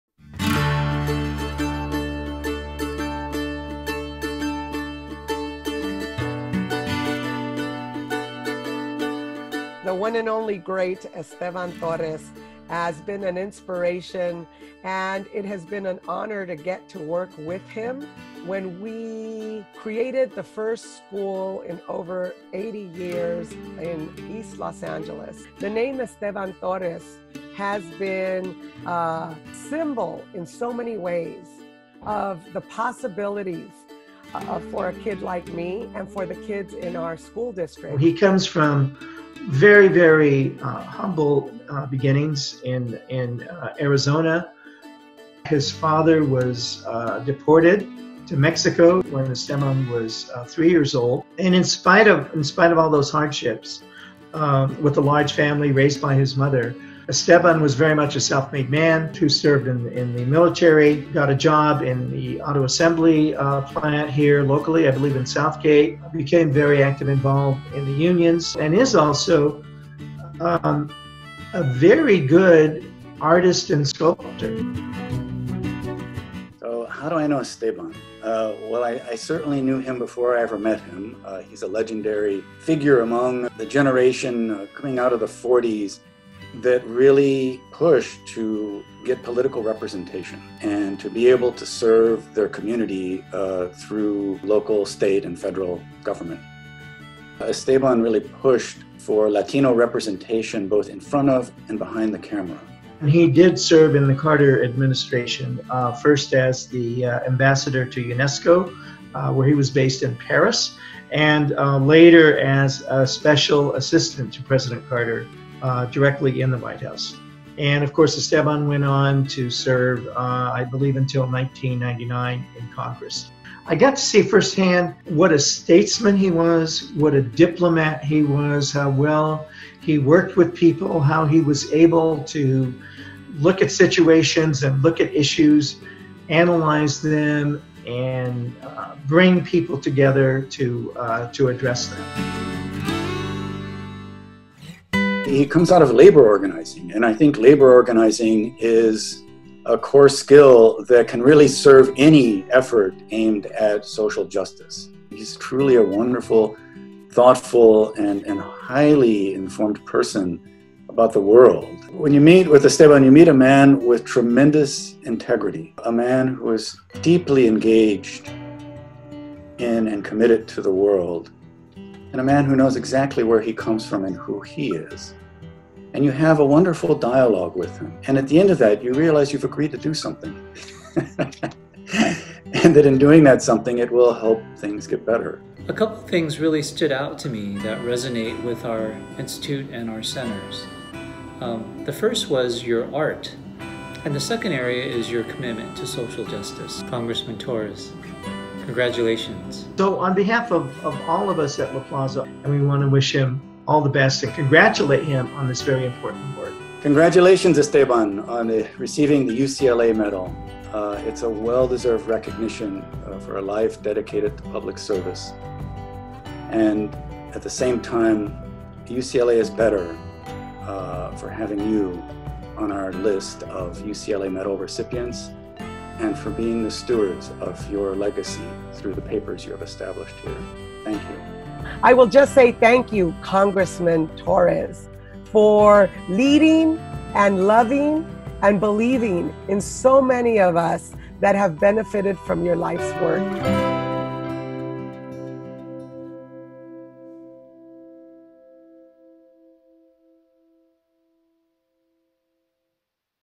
Tribute to U.S. Rep. Esteban Torres
Civic leaders and scholars offer reflections and congratulations to Esteban Torres upon his receipt of the UCLA Medal. Torres was a labor organizer and lawmaker who was among the first Latino members of Congress from California in the 20th century.